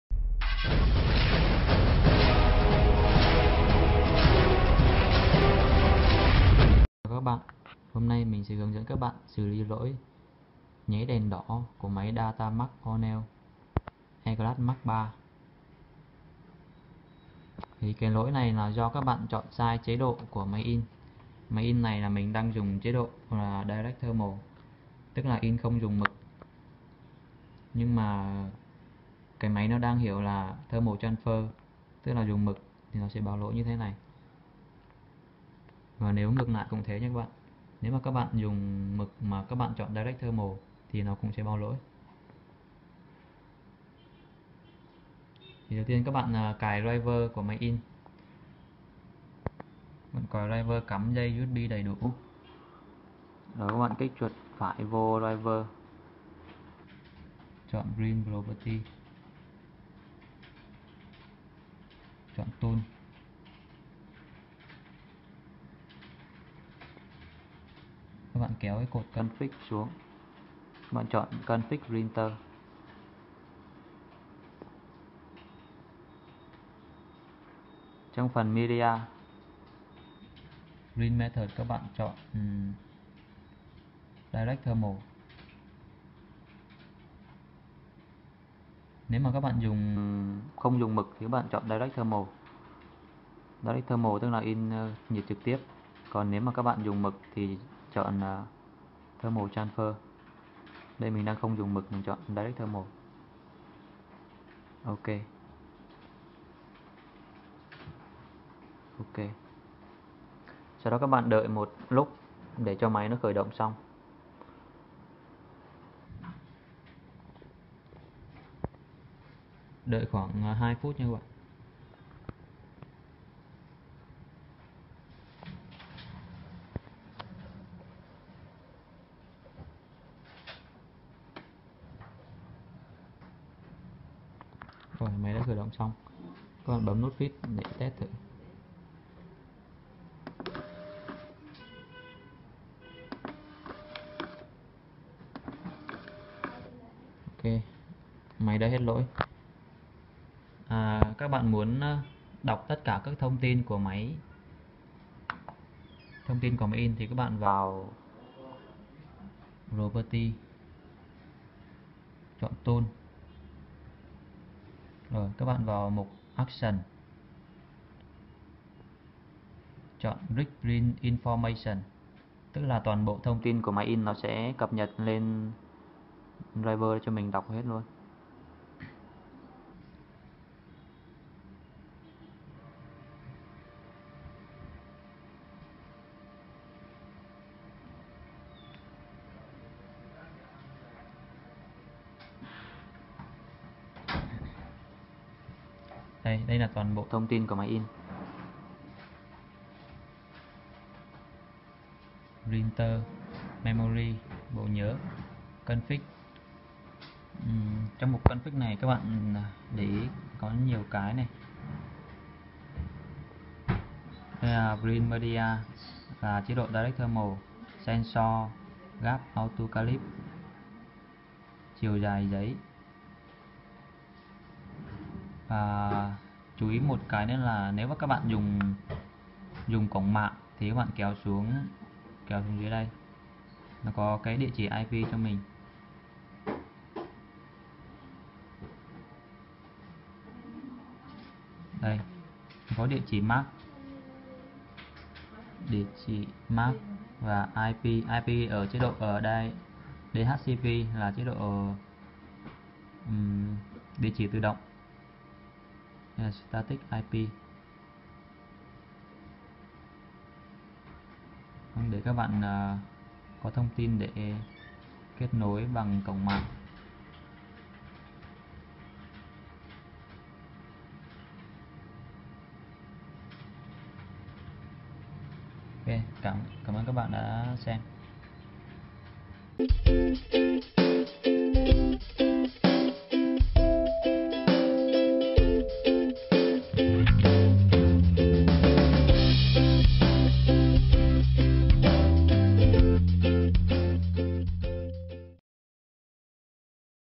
Khắc phục lỗi không hiểu giấy - Máy in Datamax-O'Neil E-Class Mark III | Công Ty Thế Giới Mã Vạch
Máy in Datamax-O'Neil E-Class Mark III khi sử dụng bị lỗi nháy đèn do máy in không hiểu giấy. Làm sao khắc phục? Khám phá ngay video!
Tìm hiểu thêm thông tin chi tiết về máy in mã vạch Datamax-O'Neil E-Class Mark III